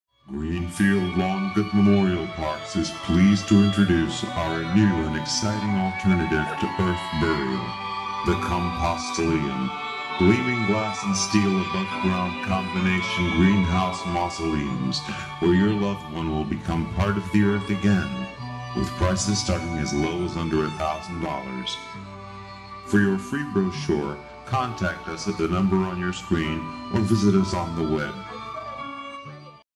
Compostoleum-Memorial Park Commercial.avi
Greenfields/Lawngoode Memorial Parks Introduce THE COMPOSTOLEUM...